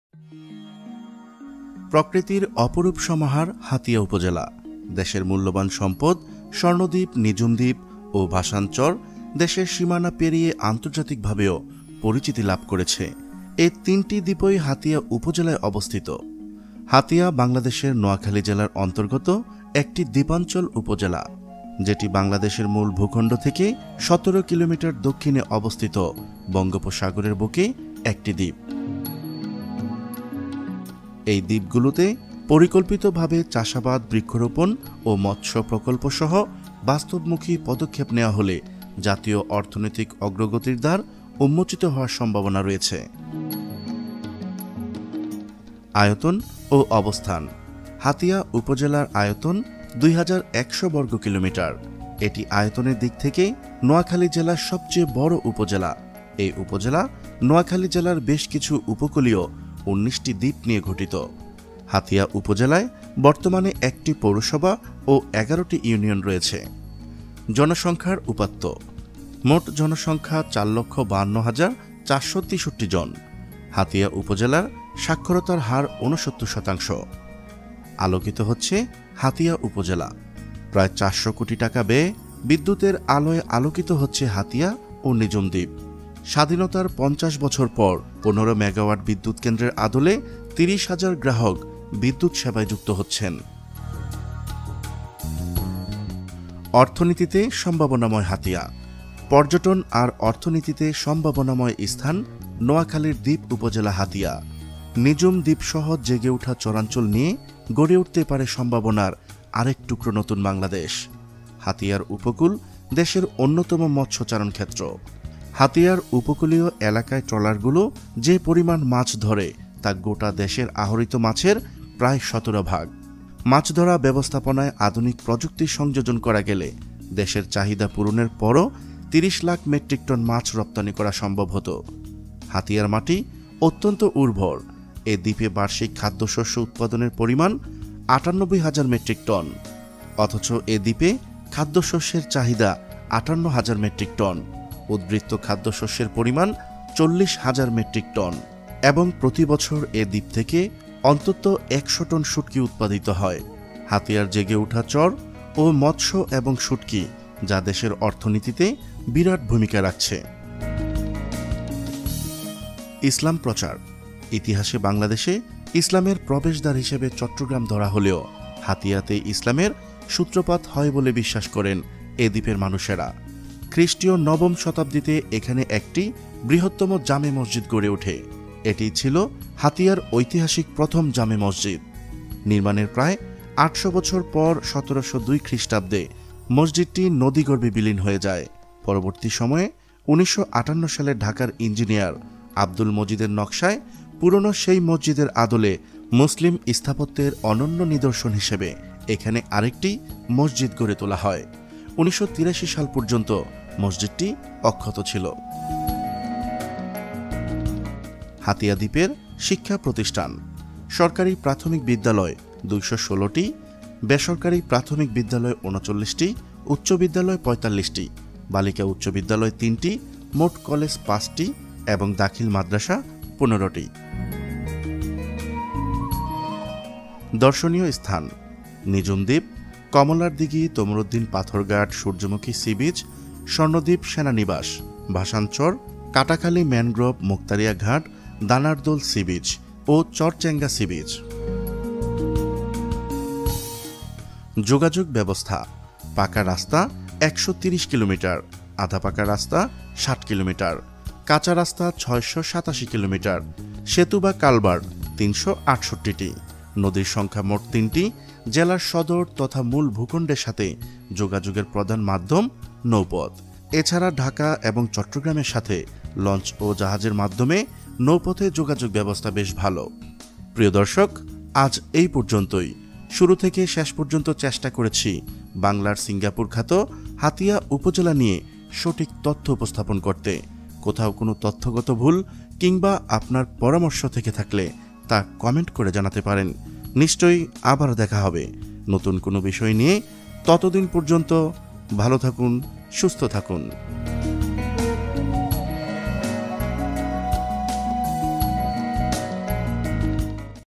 সম্ভাবনাময় হাতিয়া দ্বীপ যেন বাংলার সিঙ্গাপুর !! Documentary of Hatiya Island
প্রকৃতির অপরুপ সমাহার হাতিয়া উপজেলা। দেশের মূল্যবান সম্পদ স্বর্ণ দ্বীপ, নিঝুম দ্বীপ ও ভাসানচর দেশের সীমানা পেরিয়ে আন্তর্জাতিকভাবেও পরিচিতি লাভ করেছে। এ ৩টি দ্বীপ-ই হাতিয়া উপজেলায় অবস্থিত।
হাতিয়া বাংলাদেশের নোয়াখালী জেলার অন্তর্গত একটি দ্বীপাঞ্চল উপজেলা। যেটি বাংলাদেশের মূল ভূখন্ড থেকে ১৭ কিলোমিটার দক্ষিণে অবস্থিত বঙ্গোপসাগর এর বুকে একটি দ্বীপ।
পরিকল্পিতভাবে চাষাবাদ, বৃক্ষরোপণ ও মৎস্য প্রকল্পসহ বাস্তবমুখী পদপে নেয়া হলে জাতীয় অর্থনৈতিক অগ্রগতির দ্বার উন্মোচিত হওয়ার সম্ভবনা রয়েছে। পর্যটন আর অর্থনীতিতে সম্ভাবনাময় স্থান নোয়াখালীর দ্বীপ উপজেলা হাতিয়া।
নিঝুমদ্বীপসহ জেগে ওঠা চরাঞ্চল নিয়ে গড়ে উঠতে পারে সম্ভাবনার আরেক নতুন বাংলাদেশ। হাতিয়া উপজেলা নিয়েই আমাদের আজকের আয়োজন,